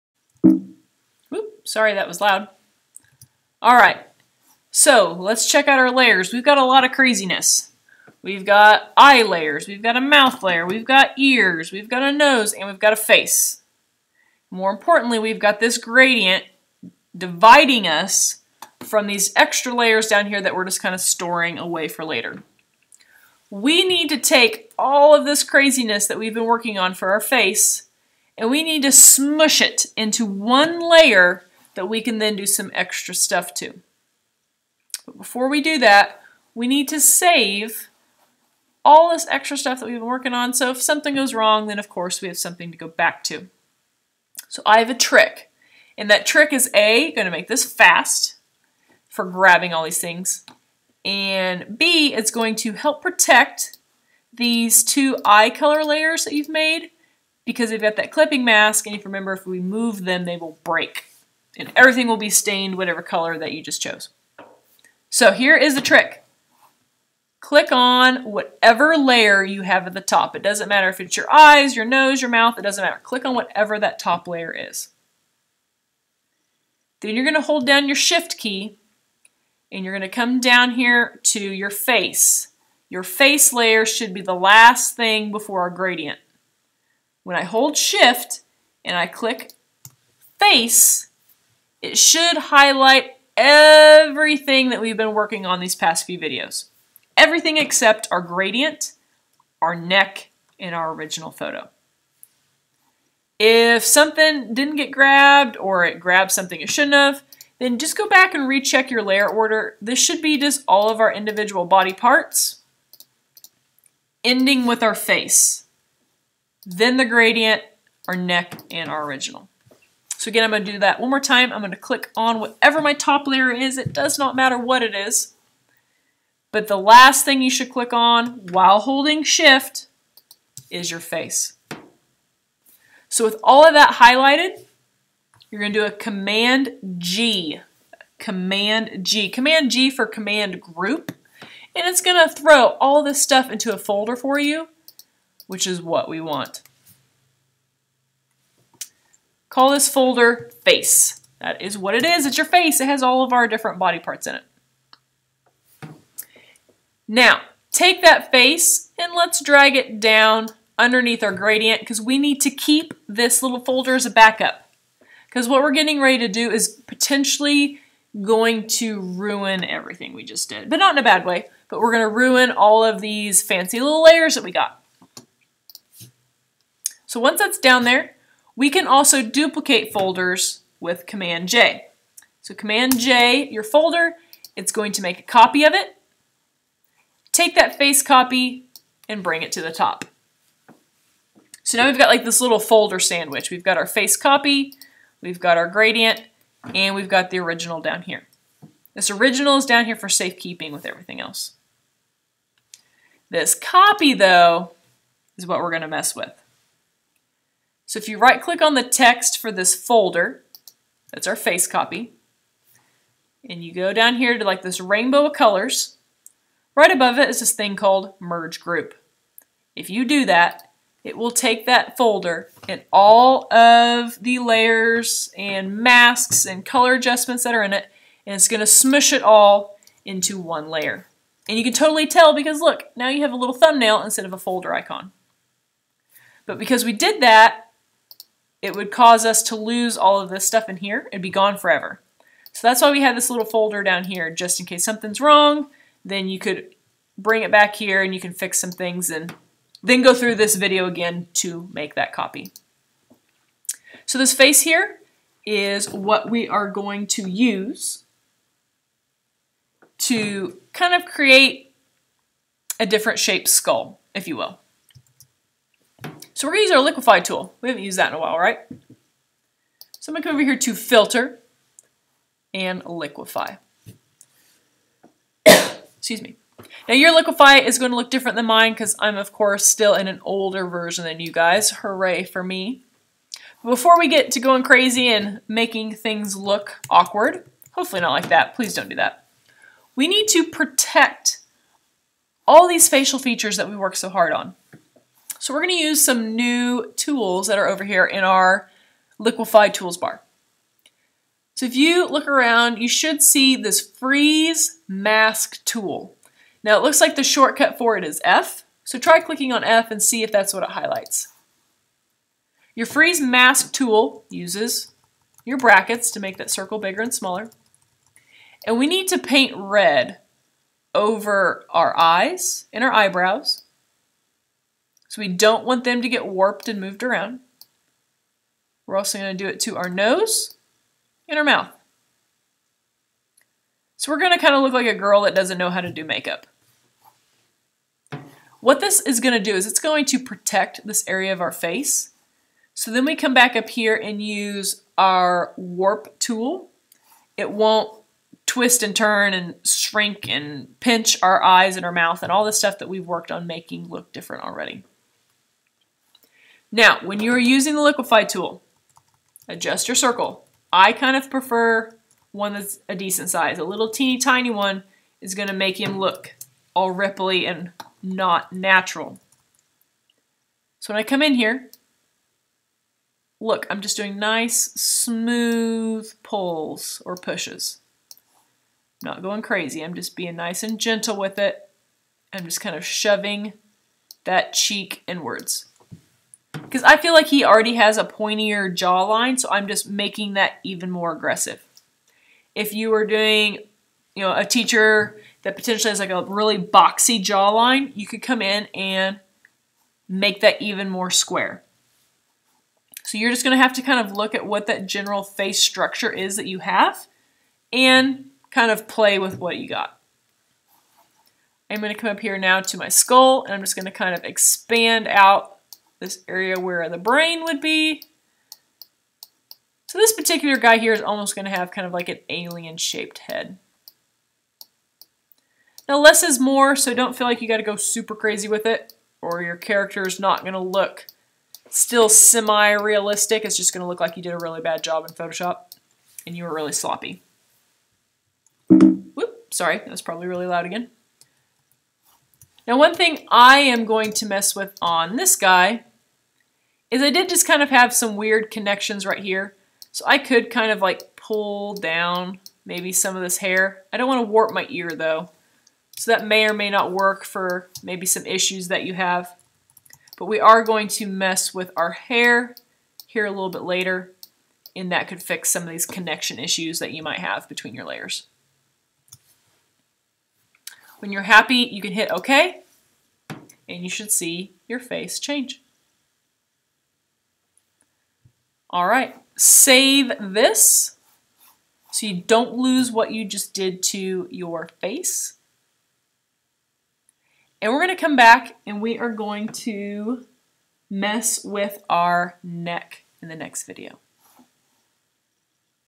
8. Merging layers and Liquifying Face
Grouping all your important face parts. Duplicating that group, and merging one of the groups into one big layer. Once the layer is created, use that layer with the liquify tool to manipulate the shape of the skull.